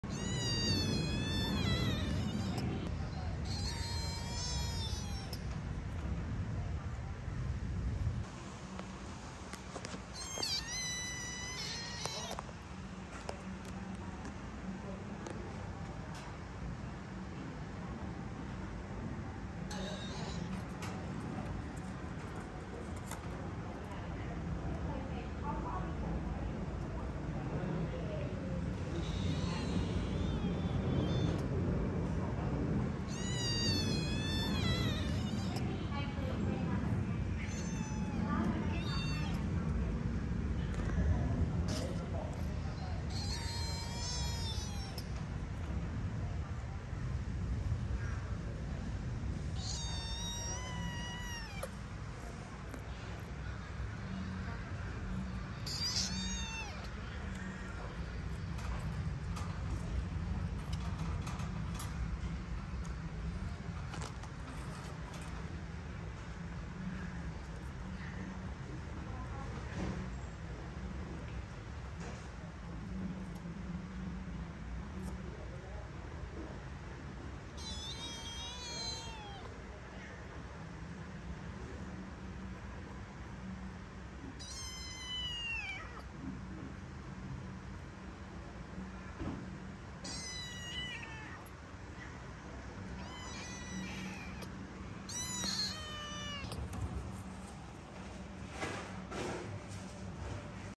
เด็กน้อยที่ตกมาจากรถเก็บขยะ ดีที่มีคนช่วยไว้ได้
📍พิกัดเหมียว : วัดบางขวาง จ.นนทบุรี
                        : Wat Bang Kwang Nonthaburi,Thailand

📸 : ธันวาคม 2566
       : December 2023